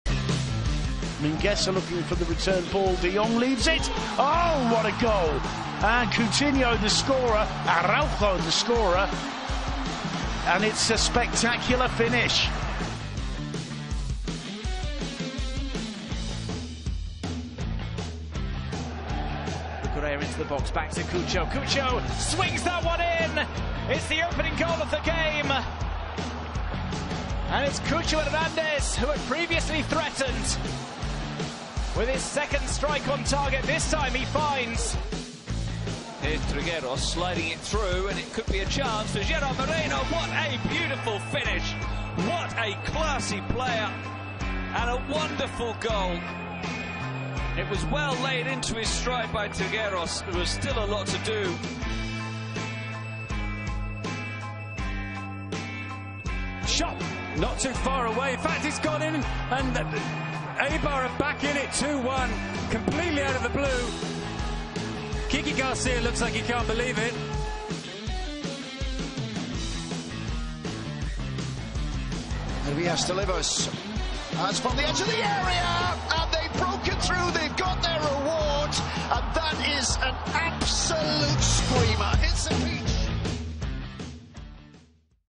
LaLiga Santander En Güzel 5 Gol | 14. Hafta 2020-2021
LaLiga'nın 14. haftasının en güzel golleri:
Ronald Araujo (Barcelona)
Cucho Hernandez (Getafe)
Gerard Moreno (Villarreal)
Kike Garcia (Eibar)
Raul Carnero (Valladolid)

LaLiga'nın hiçbir anını kaçırmamanız için Aile Spor Paketi'miz sizleri bekliyor.
Üstelik nerede izlemek isterseniz izleyin diye D-Smart GO hep yanınızda!